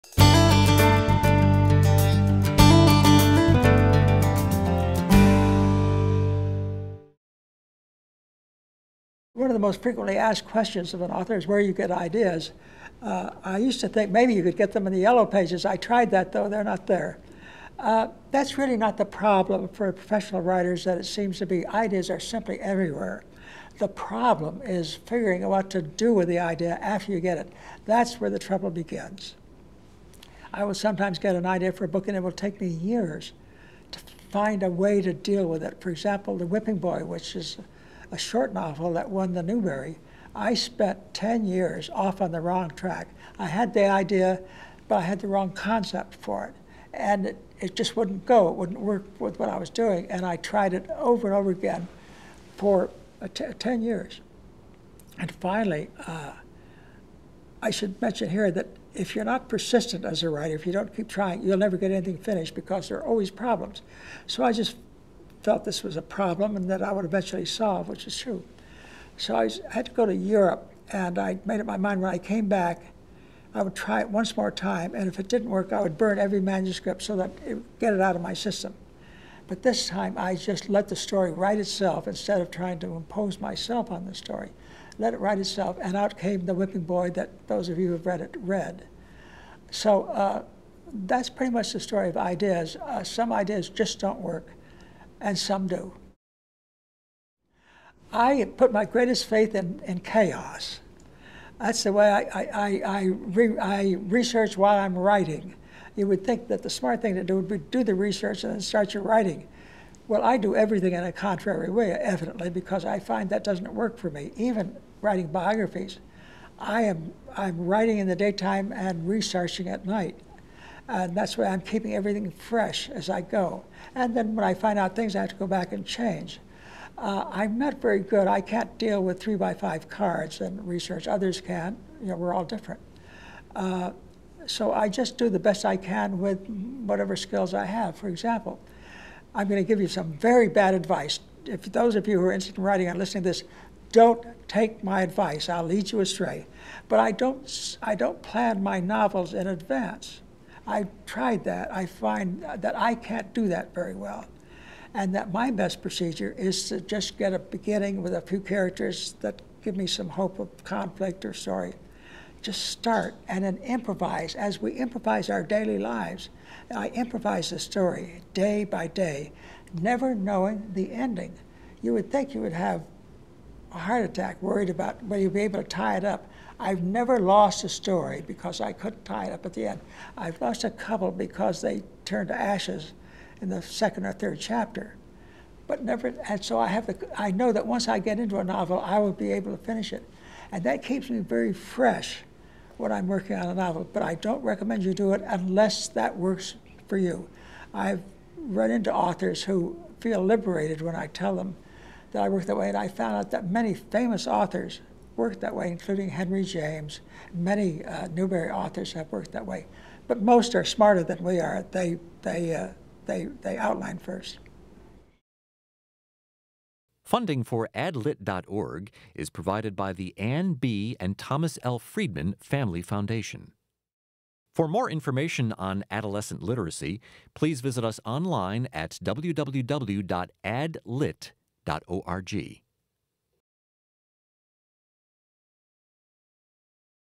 Meet the Author: Sid Fleishman
What's the trick to writing more than 60 books? Award-winning author Sid Fleischman found the magic of writing a book is well, like magic. Fleischman toured as a professional vaudeville magician after he finished high school and wrote several books about magic before he realized that a mystery novel is very like a magic trick, only the author is kind enough to explain what's happened where a magician keeps his secrets to himself.